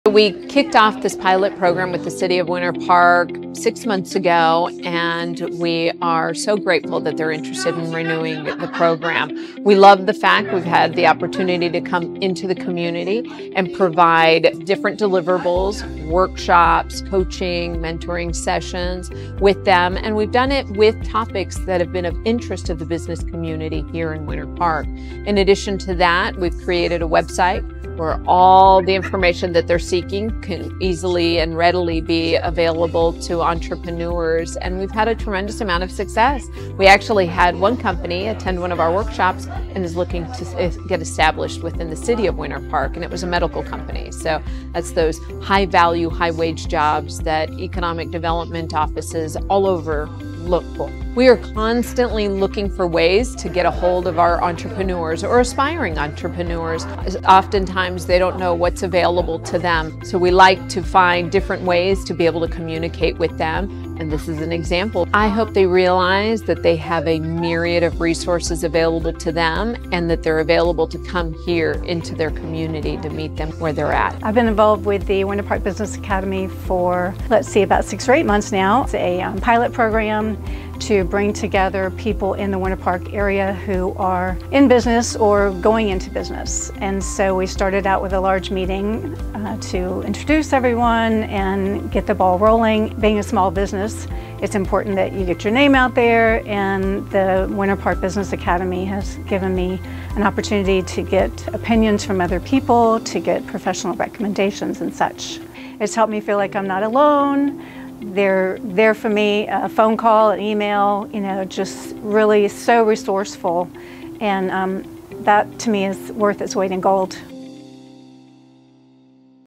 NEC & Winter Park Business Academy: GROW!!
The NEC and Winter Park Business Academy has been an incredible success!

152 participants.
More than 1,000 visitors to the website...and 8,500 social media impressions!

This is how cooperation powers community!